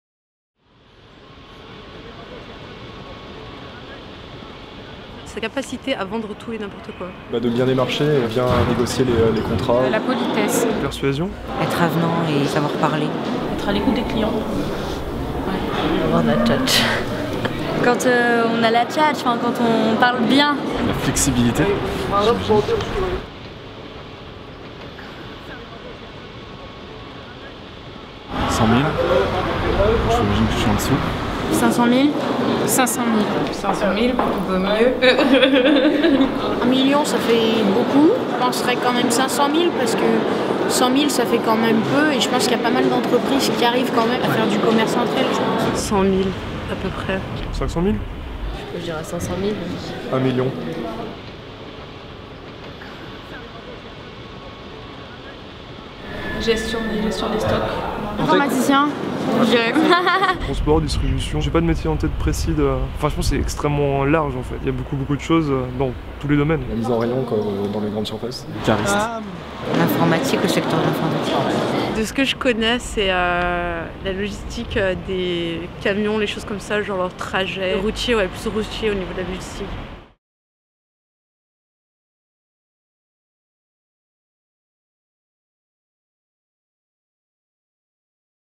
Le commerce BtoB et vous !?
Dans le cadre de l'lnfolab du Mondial des Métiers 2017, nous avons interrogé des jeunes de l'agglomération lyonnaise sur leur vision des métiers du commerce BtoB.

En partenariat avec le Mondial des Métiers et les métiers du commerce BtoB.